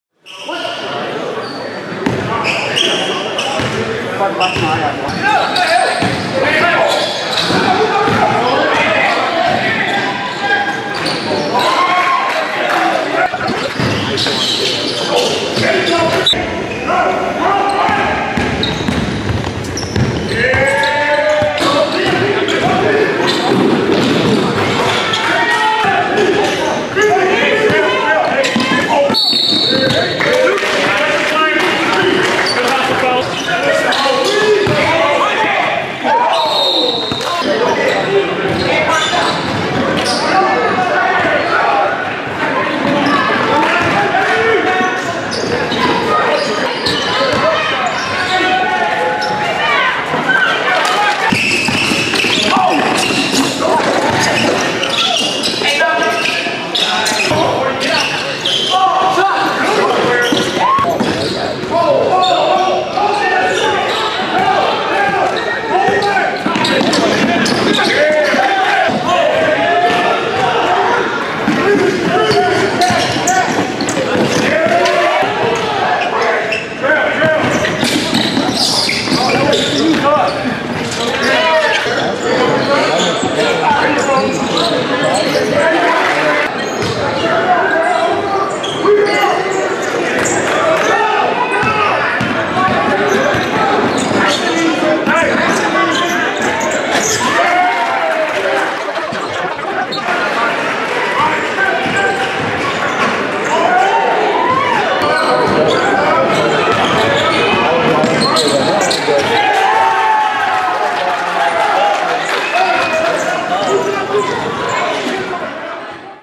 2015 Forward Nick Blair Has MADE IMPROVEMENTS!!! Adidas Invitational Highlights Dream Vision AAU
Here's Nick Blair showing he can improve in a months time.